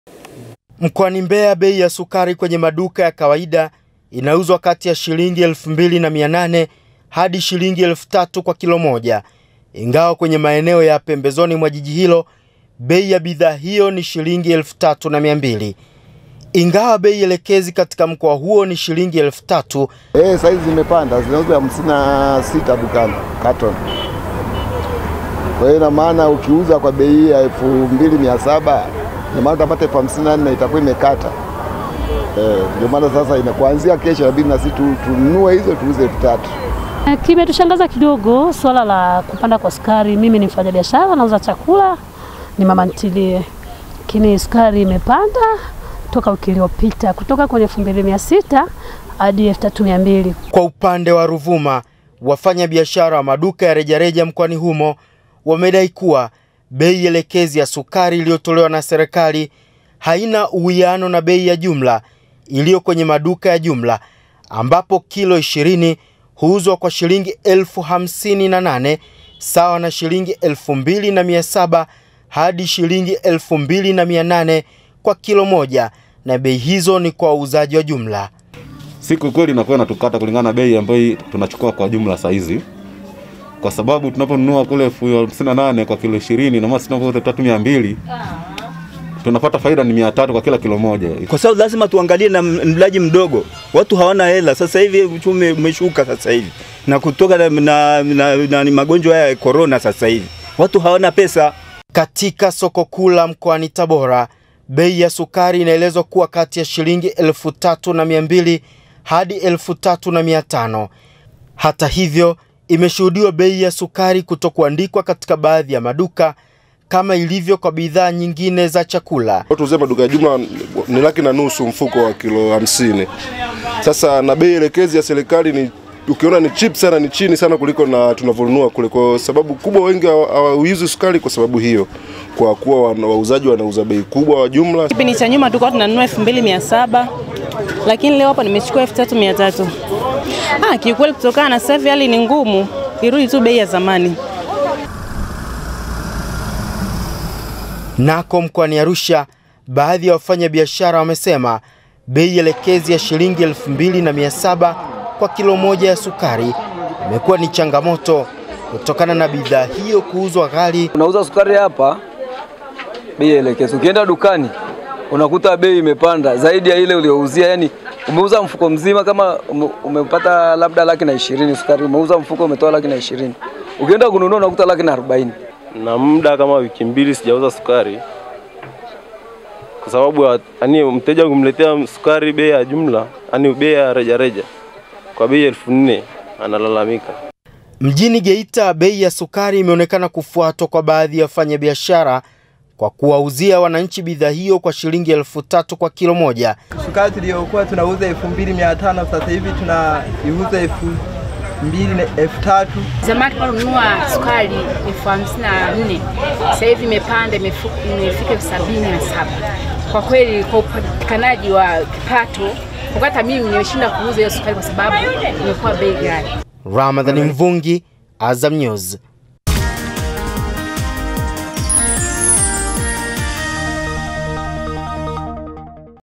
Mtazamo wa wananchi juu ya bei elekezi ya sukari
Wakati Serikali ikiweka bei Kikomo katika kudhibiti upandishaji holela wa Sukari hasa katika kipindi hiki dunia ikipambana na janga la Corona, wafanyabiashara pamoja na wanunuzi wamekuwa na mitazamo tofauti juu ya msimamo huo wa Serikali.